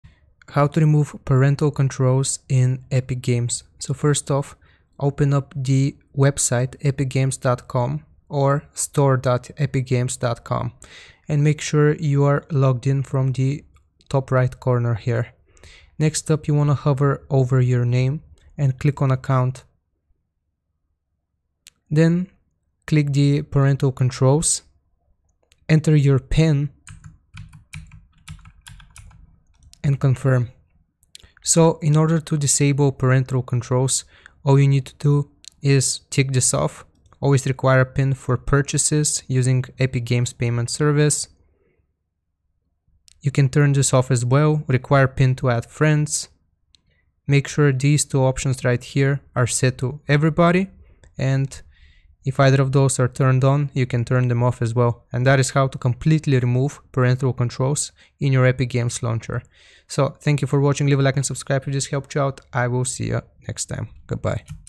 How To Remove Parental Controls From Epic Games
In this video I show you how disable and remove parental controls altogether from Epic Games! If you want to tamper with your Epic Games settings and turn off Parental Controls (pin) then this is the video for you! Removing parental controls from Epic Games has never been easier!